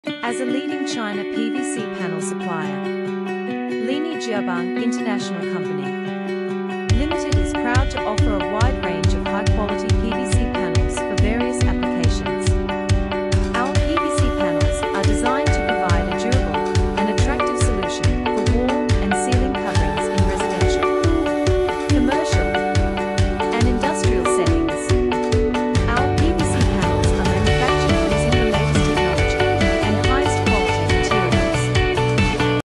China Pvc Panel Supplier
As a leading China PVC panel supplier. Linyi Jiabang International Co.. Ltd. is proud to offer a wide range of high-quality PVC panels for various applications. Our PVC panels are designed to provide a durable and attractive solution for wall and ceiling coverings in residential. commercial. and industrial settings. Our PVC panels are manufactured using the latest technology and highest quality materials.